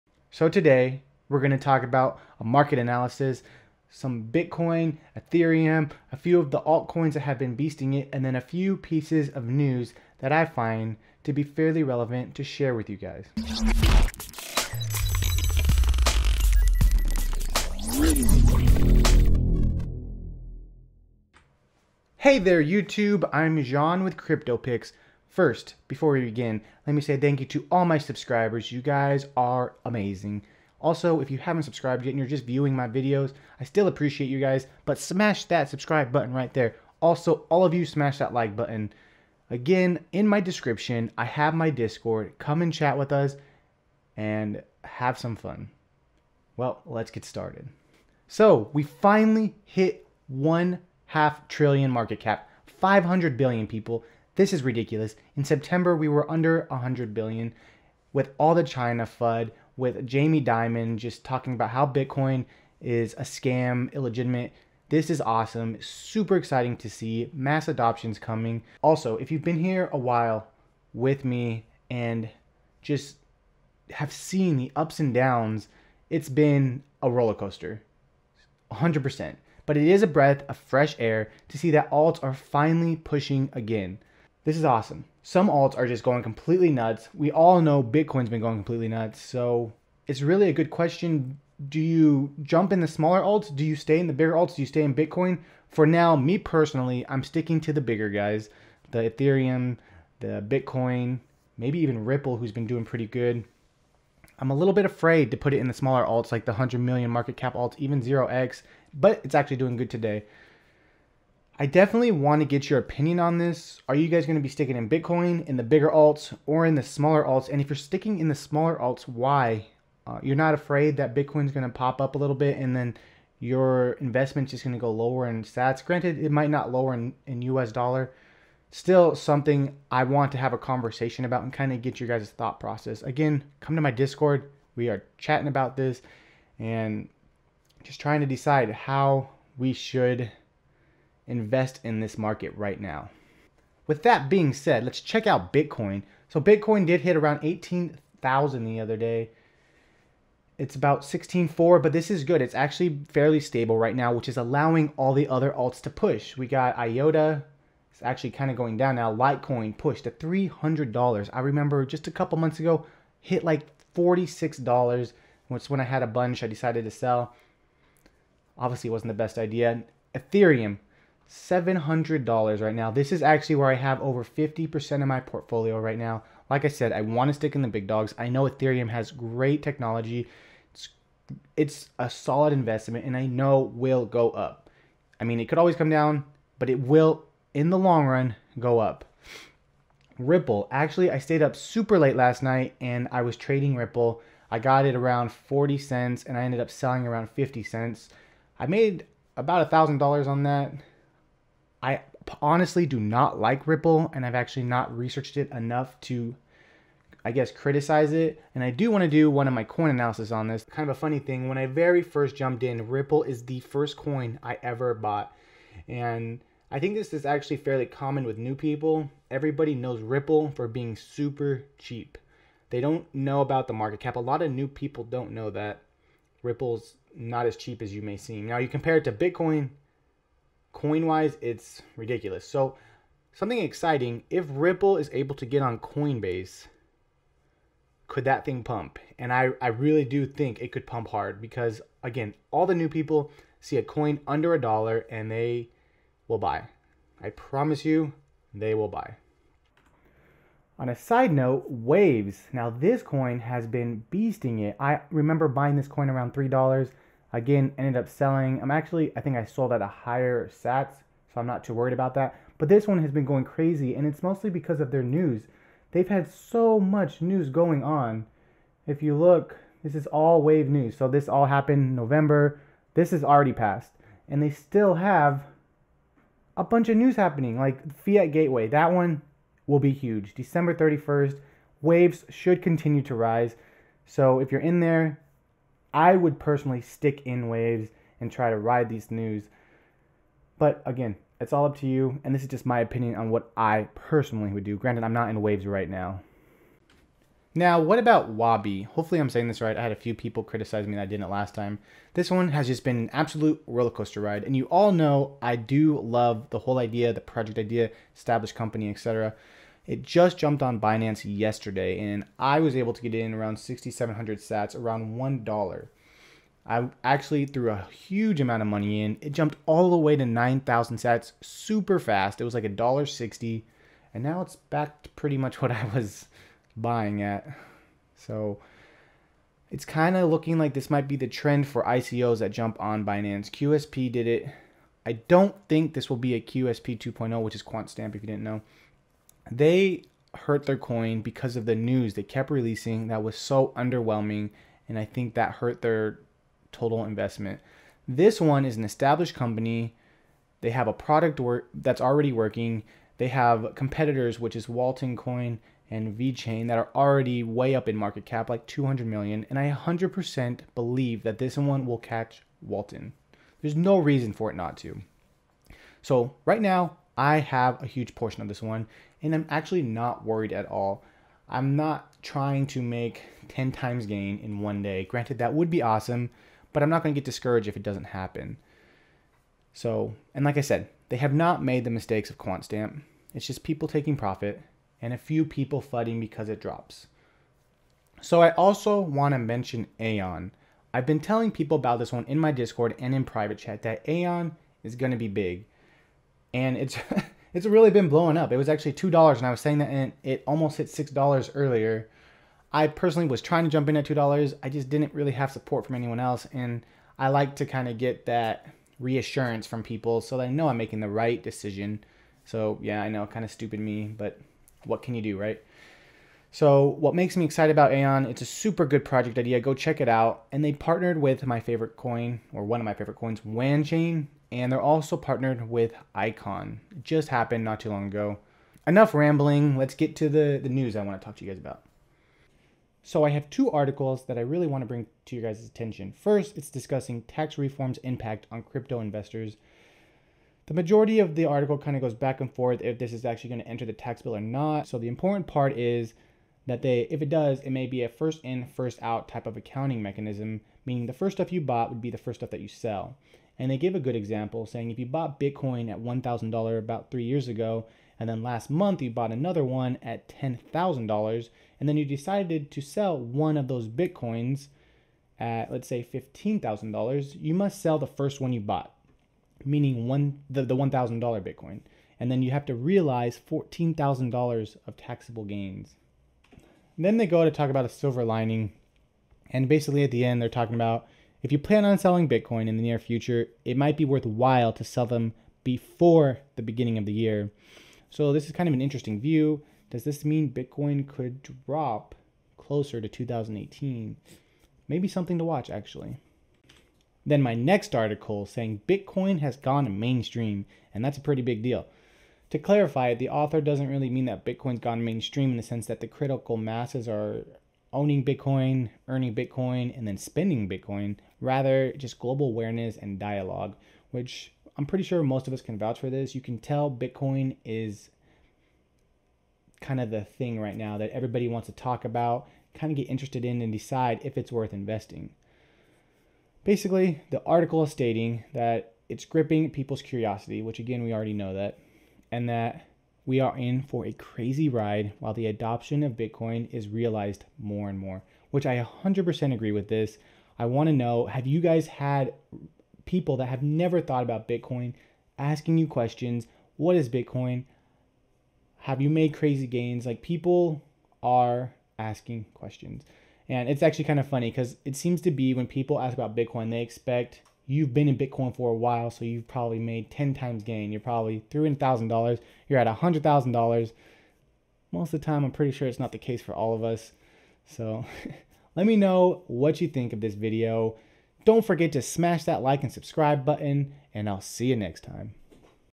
Half Trillion Market Cap | Alts | Crypto News
This video is a quick talk about the exploding market cap, a few alt coins and some crypto news.

🌟Smartcash Address: SVRzuacA3meukaVRwMfDXezepChES42Ydz

Suggestions and feedback are always welcomed!